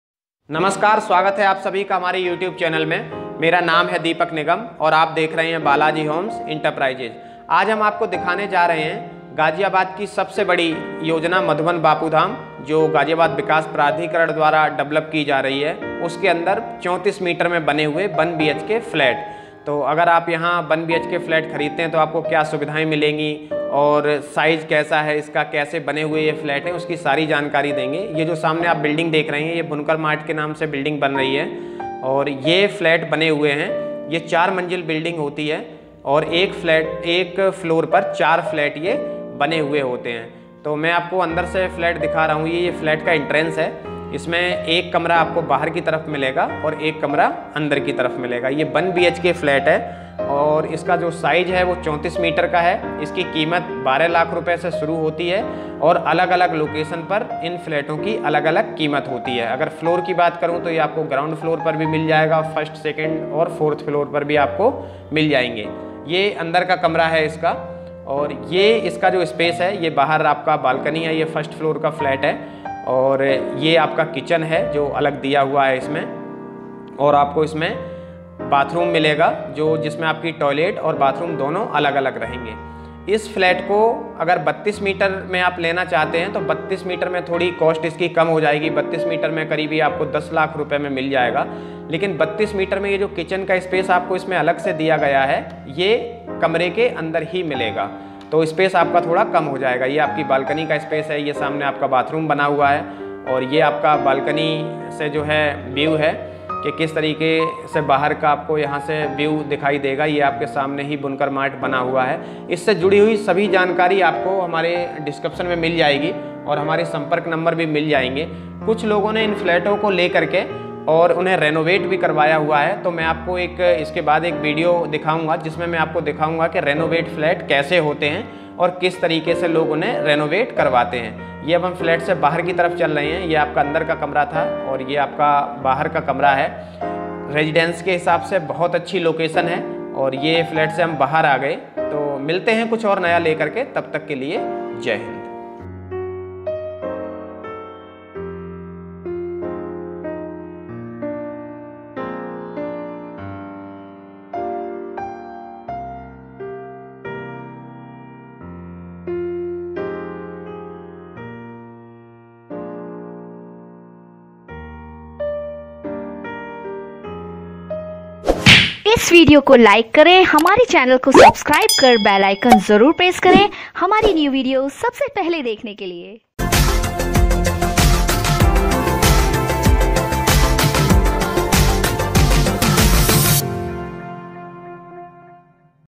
गाजियाबाद में खरीदें सबसे सस्ते फ्लैट | मधुबन बापूधाम योजना | G D A Approved | 32 और 34 मीटर |Flat
योजना का नाम - मधुबन बापूधाम आवासीय योजना गाजियाबाद (क्षेत्र गोविंदपुरम)

यह योजना गाजियाबाद विकास प्राधिकरण के द्धारा विकशित की जा रही है जो करीब 1400 एकड़ में फैली है।

फ्लैट का साइज - 32 मीटर (10  लाख),34 मीटर (12 लाख),40 मीटर,60 मीटर (22 लाख)

प्लाट साइज - 60 मीटर (25 लाख),90 मीटर (35 लाख),112 मीटर(45  लाख),150 मीटर (55 लाख),200 मीटर (65 लाख)

नोट : गाजियाबाद विकाश प्राधिकरण की बड़ी हुई कीमत की दरें इस कीमत में शामिल नहीं है।

प्रॉपर्टी की कीमत लोकेशन के अनुसार अगल - 2 है।

इस पूरी योजना को अलग - 2  ब्लॉक में बाँटा गया है।

इस योजना में आपको सभी सुविधाये मिलेंगी जैसे - पार्क,सीवर,पानी की सप्लाई,हॉस्पिटल,सरकारी स्कूल,बैंक,शॉपिंग मॉल,पेट्रोल पंप,बाजार,पुलिस चौकी,आपके घर से पैदल की दूरी पर।

गाजियाबाद पुलिस लाइन,P A C लाइन,N D R F बटालियन,कलेक्ट्रेट,एस एस पी ऑफिस 1 से 2 किलोमीटर की दूरी पर।

ईस्टर्न पेरिफेरल एक्सप्रेस वे और दिल्ली मेरठ एक्सप्रेस वे 5 मिनट की दूरी पर।

आने वाले समय में गाजियाबाद विकास प्राधिकरण का ऑफिस यहीं शिफ्ट होगा।

रोड साइज - 9 मीटर ,12 मीटर,18 मीटर

NH - 58 से 3 मिनट की दूरी

NH - 24  से 5 मिनट की दूरी

सरकारी बैंक से लोन

हमें फेसबुक पर भी फॉलो करें - @balaji9953

 SCHOOLS / COLLEGES IN VICINITY
 Sunder Deep Group Of Institution.
 Adhyatmik Nagar Inter Collage.
 Rama University.
 I M S Ghaziabad.
 Radicon School.
 The DPSG International.
 Sunder Deep World School .
 Gurukul The School NH-24.
 Kendriya Vidyalaya .

   HOSPITALS IN VICINITY
 Columbia Asia Hospital NH – 24.
 Maxwell Multi Specialty Hospital.
 Shivalik Hospital.

      CLOSE PROXIMITY  (DRIVING DISTANCE)
 NH-24 ( 3 Min.)
 NH – 58 ( 5 Min.)
 Delhi – Meerut Expressway ( 5 Min.)
 C.B.I. Academy Ghaziabad (10 Min.)
 Bus Stand (10 Min.)
 Ghaziabad  Metro Station (15 Min.)
 Ghaziabad Railway Station – 7 K.M
 New Ghaziabad Railway Station – 3 K.M
 Old Bus Stand – 4 K.M
 CGO Complex – 2 K.M
 Hindon Air Force – 12 K.M
 Mohan Nagar – 10 K.M
 Anand Vihar (ISBT) 18 K.M